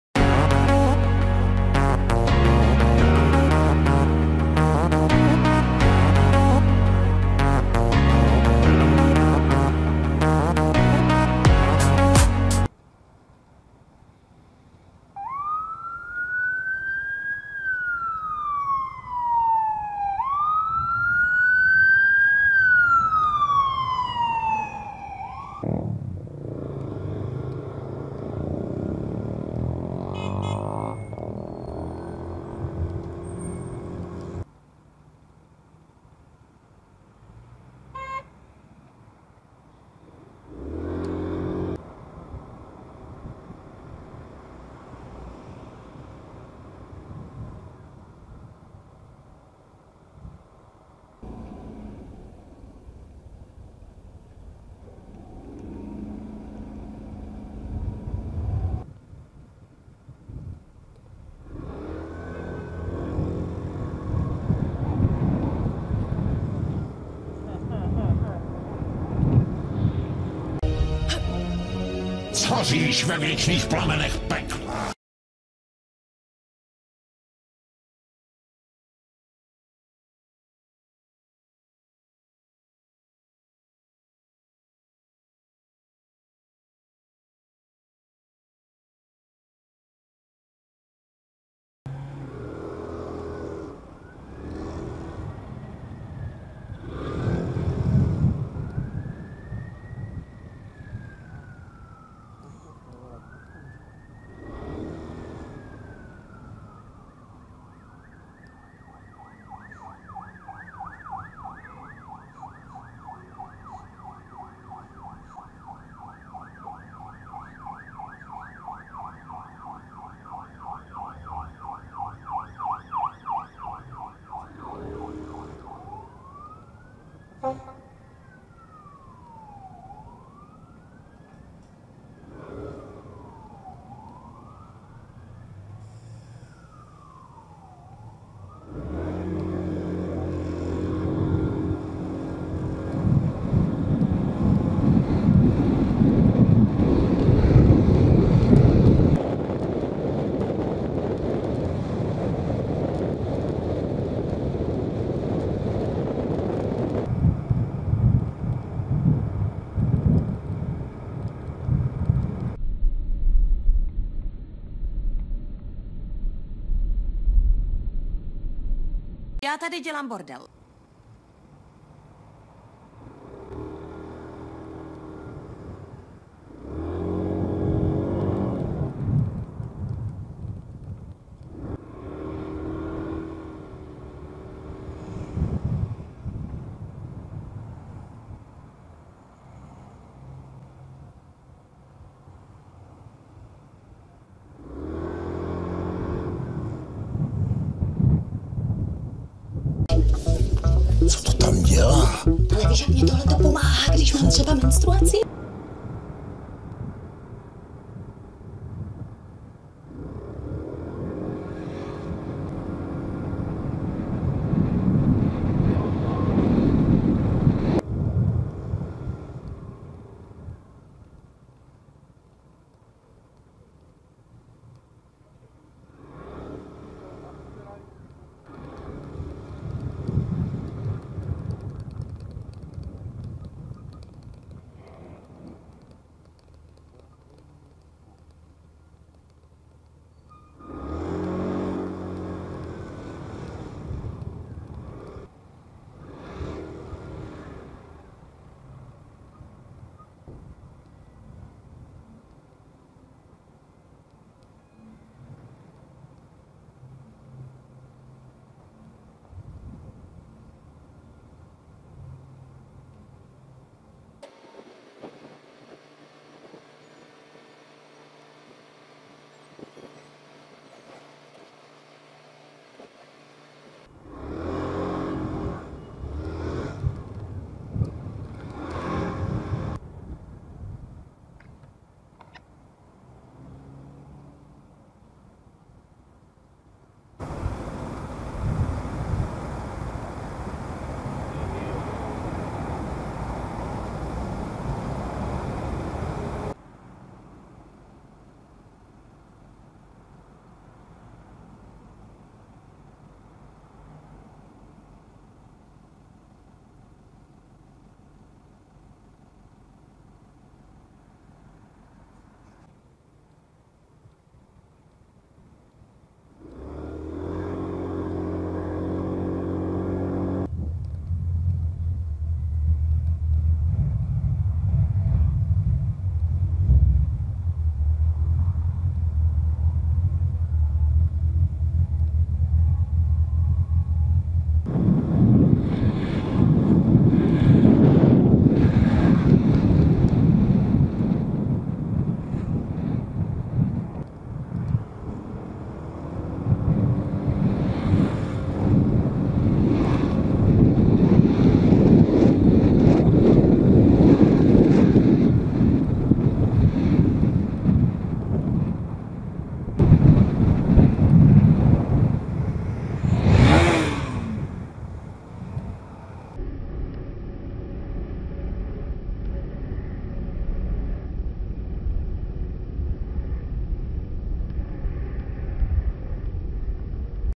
Prý jsem DEBIL, Blokoval 🚑, Zacpali křižovatku, Dělají bordel, BMW se toho bojí - SkútrCam #59
Dnes uvidíte:
0:00 Intro
0:12 Blokování sanitky
0:26 Nedání přednosti
0:34 Nedání přednosti II
0:42 Nechtěl pustit kolo
0:51 Stání na přechodě
0:59 Stání v křižovatce
1:15 Nová autobusová zastávka
1:32 BMW
2:29 Nebezpečné předjíždění
2:37 Traktor dělá bordel
2:50 Uvíznutí v křižovatce
3:02 Manévr na Chocholouška
3:25 Špatná směrovka
3:38 Česká pošta
4:40 Jsem prý debil
4:57 Bez odhadu
5:13 Červená
5:20 Policie
5:34 Jeduuu
5:44 Motorkářka
6:07 Fanoušci